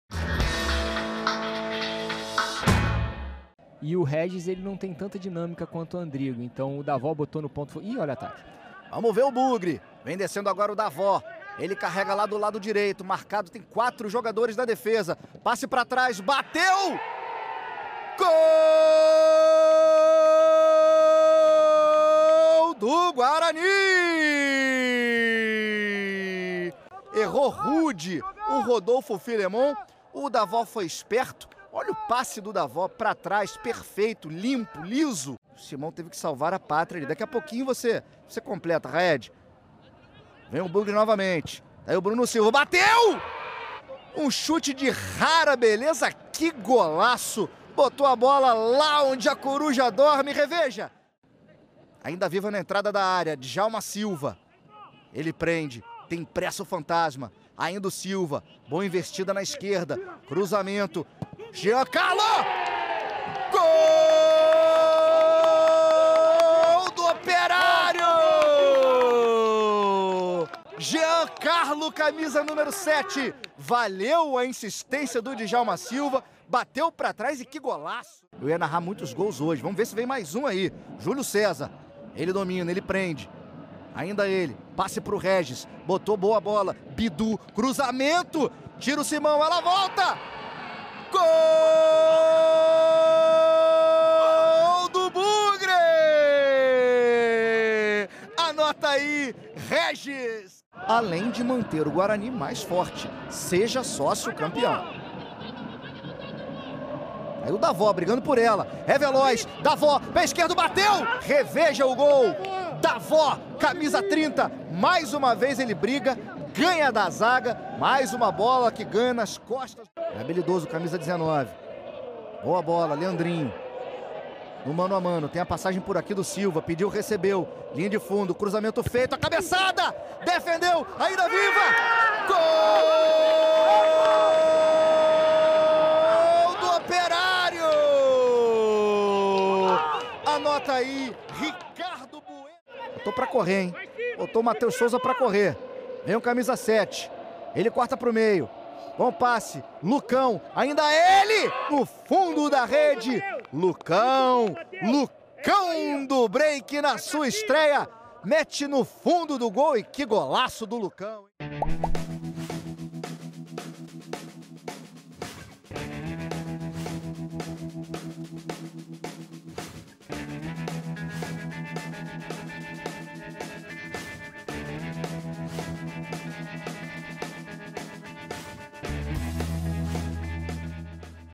OPERÁRIO-PR 2 X 5 GUARANI | GOLS | 2ª RODADA BRASILEIRÃO SÉRIE B 2021 | ge.globo
Em jogo de sete gols, Guarani vence o Operário-PR e dorme na liderança da Série B. Operário-PR e Guarani fazem duelo eletrizante, com sete gols, um gol mal anulado e duas expulsões no Couto Pereira; Bugre sobe para primeiro, e Fantasma cai para sexto